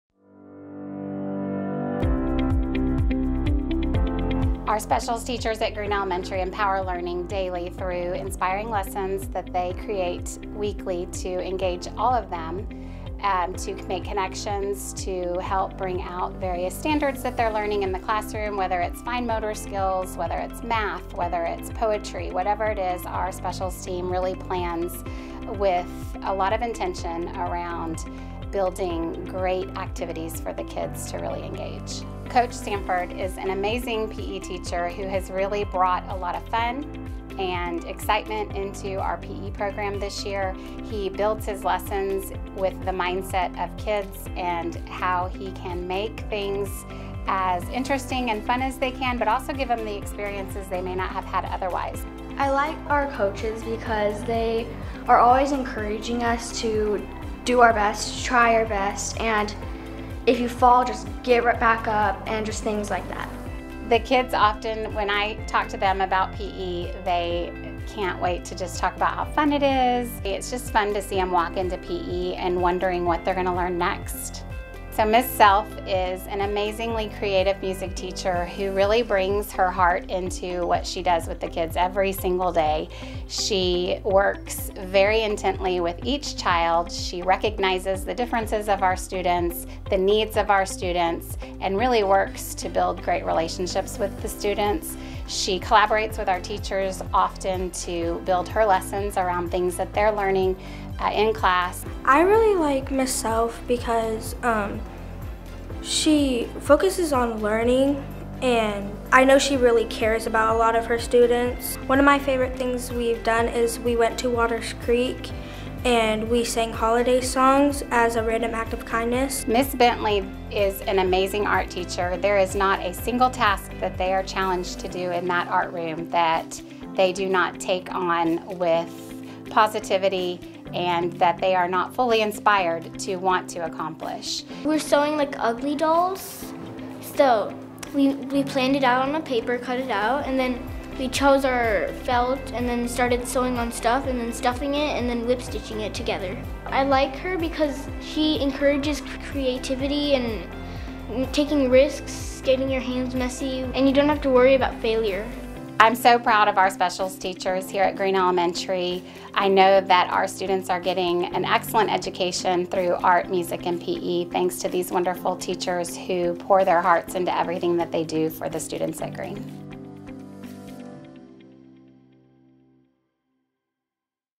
Empowered Learning May 2023 Green Elementary
The Specials Team, Aerie Bentley (Art), Rhonda Selph (Music), and Chad Sanford (PE), at Green Elementary fosters an environment where our students are ignited in their curiosity and empowered in their learning. Each department takes time to create engaging and meaningful lessons that allow students to explore each of these essential areas.